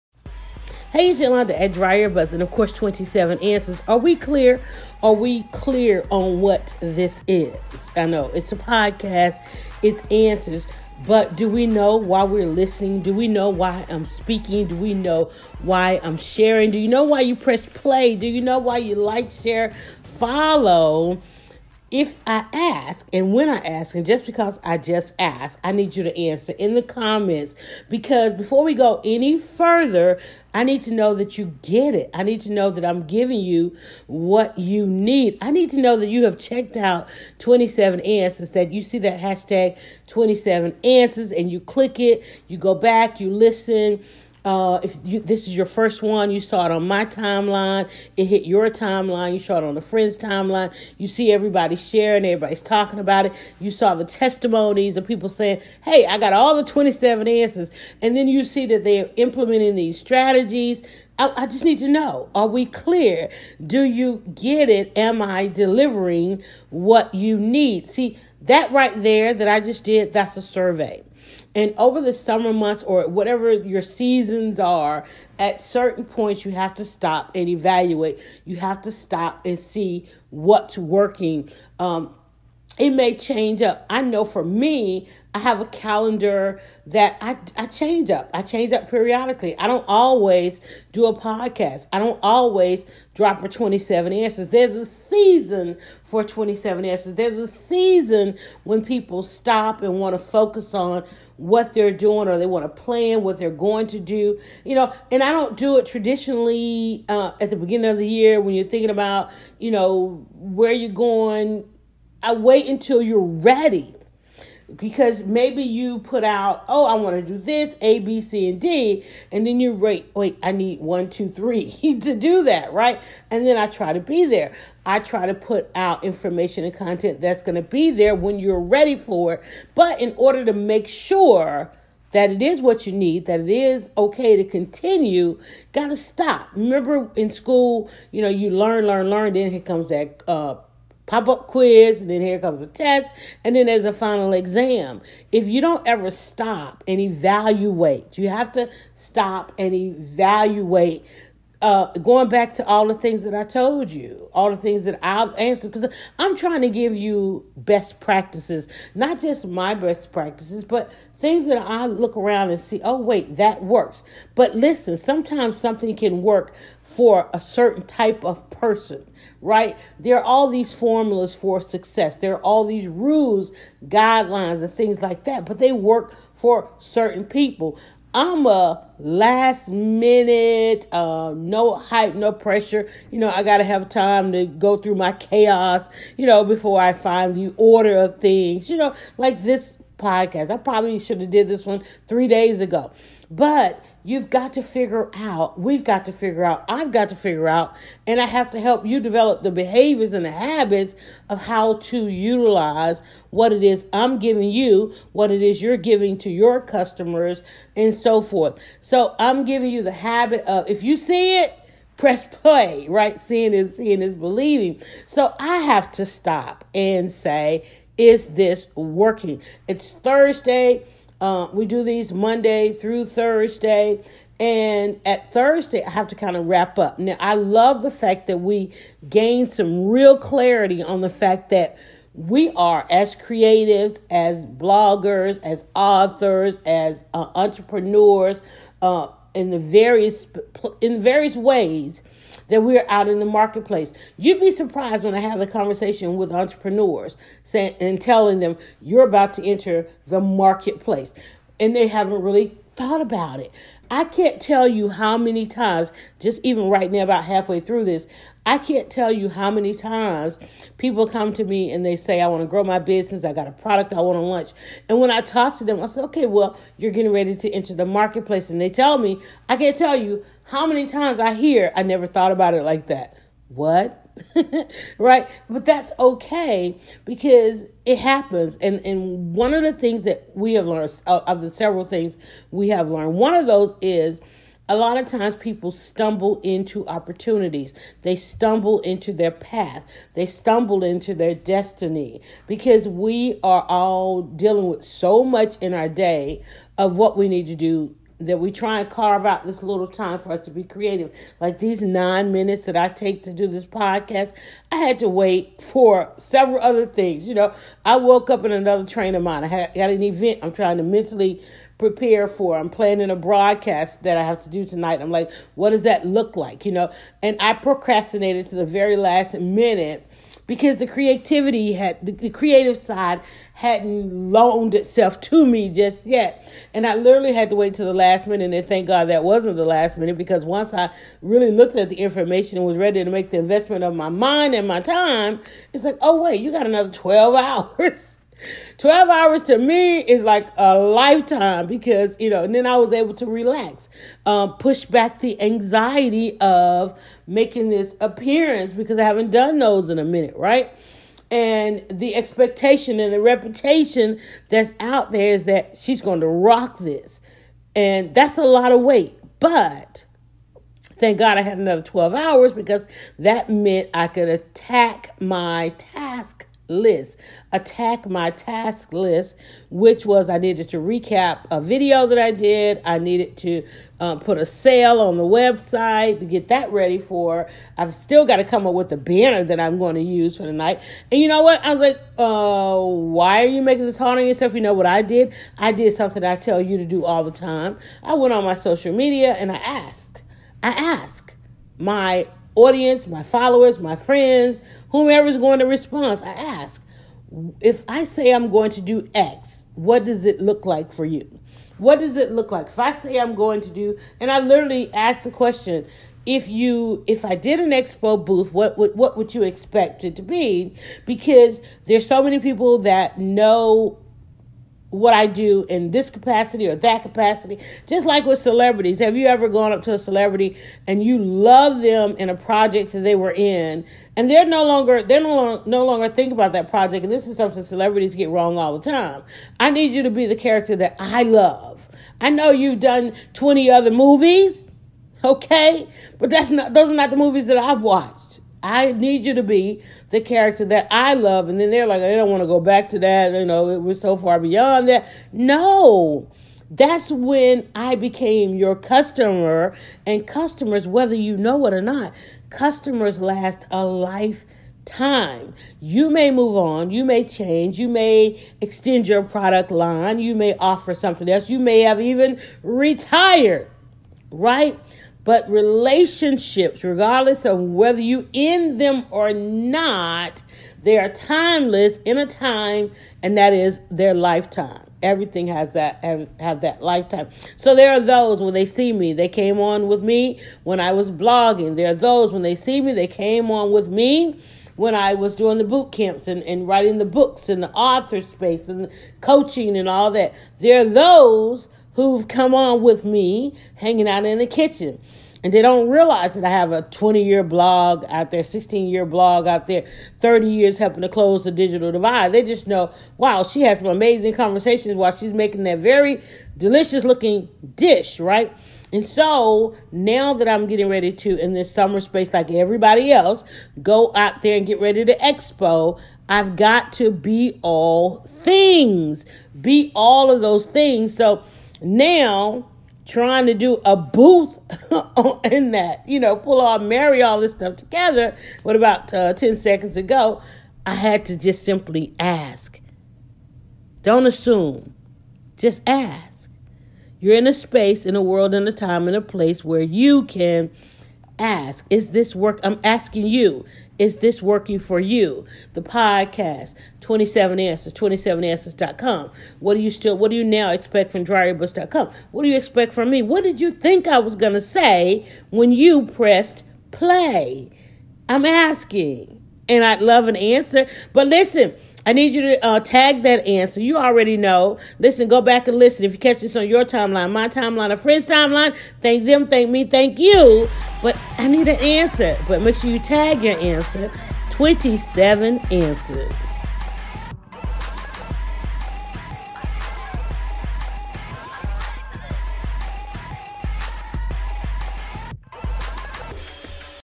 How to Create Buzz | Ask Don’t Tell | Answer of the Day
Listen creatives listen ... Don’t assume. Just ask!

Let us all learn from IHOP. Before you touch your brand, don’t assume — just ask!

So we’re asking. Is this podcast everything we promised it would be? Are our answers working for you as a creative being self employed or working on your side hustle? Is the podcast helping you find you space in the marketplace?

Listen creatives listen ... to another episode of How to Create Buzz by DryerBuzz Yalanda Lattimore blogger author creative business success poscast DryerBuzz 27Answers 27Answers.com